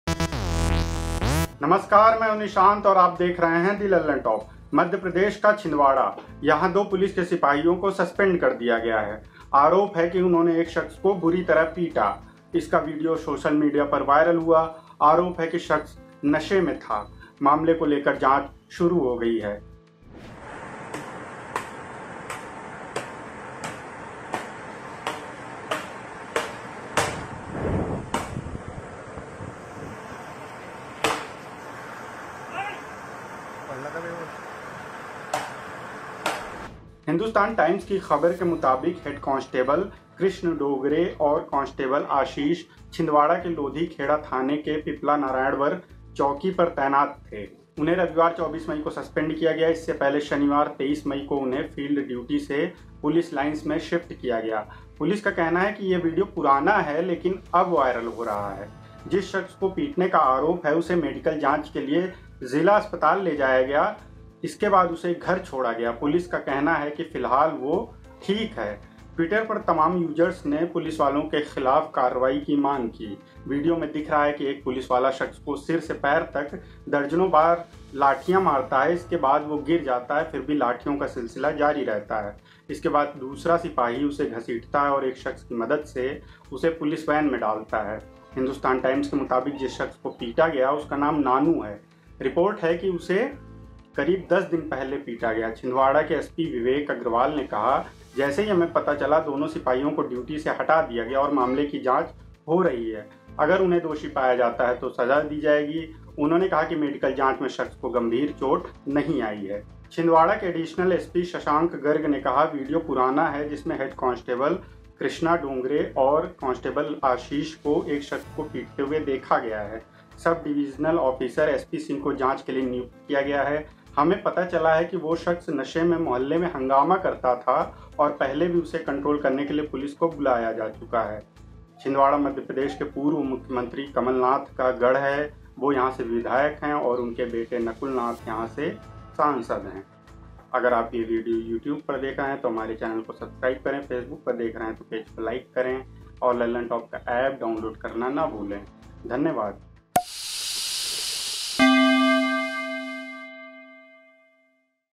MP में एक शख्स को बुरी तरह पीटने के आरोप में दो पुलिसवाले सस्पेंड हो गए, Video Viral
A video of two policemen beating a man brutally in Chhindwara went viral after which they were suspended and a probe has been started. Head constable Krishna Dingre and constable Ashish posted at Piplyanarayanwar police outpost under Lodhikheda police station in Chhindwara. The man who was allegedly beaten up was taken to the district hospital for medical examination after which he was dropped at his residence. His condition is fine, said police.

The man who was thrashed, has been identified as Nanu. ASP, Chhindwara, Shashank Garg said, The video is old in which head constable Krishna Dongre and constable Ashish were seen beating a man.

SP, Chhindwara, Vivek Agrawal said, “The incident is unfortunate. As we came to know about it, I immediately suspended the police personnel and ordered an inquiry into the incident. There will be exemplary punishment against them if they are found guilty in the inquiry.”